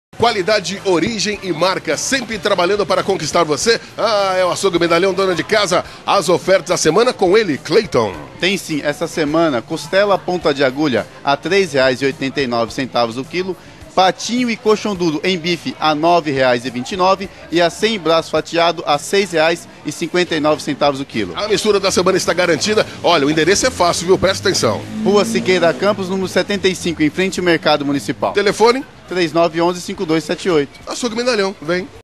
Vale Shop - Programa 42 - Açougue Medalhão SJC
Comercial produzido pela equipe Vale Shop e veiculado no canal Band Vale, para o cliente Açougue Medalhão SJC